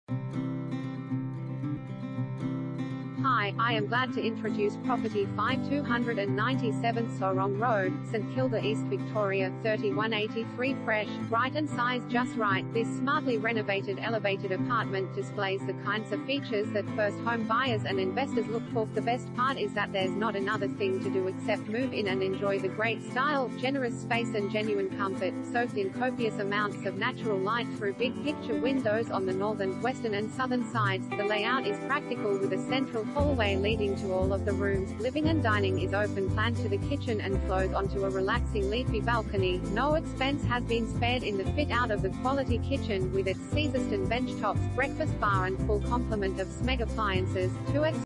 5/297 Orrong Road, St Kilda East VIC 3183 | AuPropertyReport.Com
5/297 Orrong Road, St Kilda East VIC 3183
Property Type: apartment
Bedrooms: 2
Bathrooms: 2
Car Space: 1
Fresh, Bright And Sized Just Right